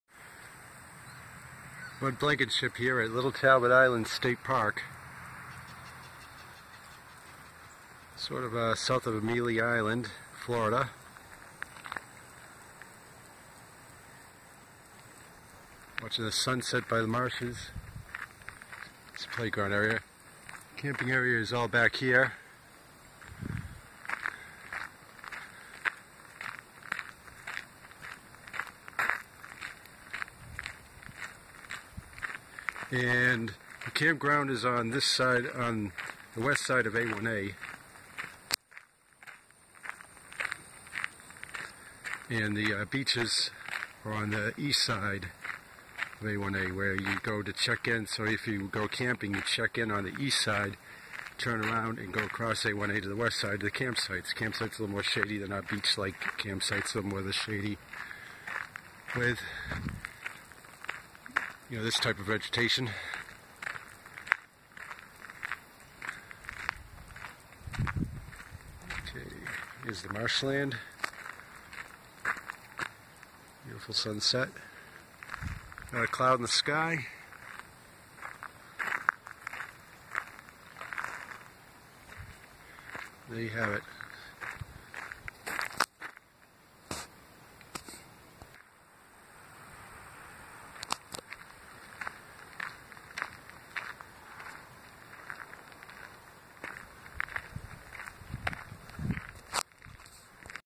Sunset: Little Talbot Island State Park
Sunset over the marshland at the campground side at Little Talbot Island State Park near Amelia Island Florida.